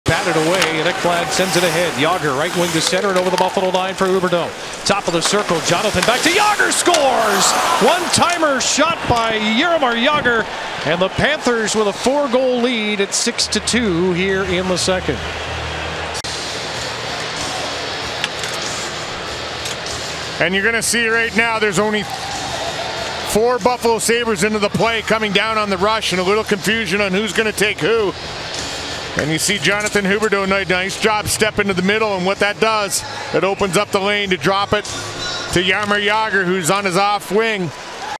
Gotta See It: Jagr scores best beer league goal NHL has ever seen
Watch as Jaromir Jagr takes a pass, creates a lane to the net and deposits a one-timer.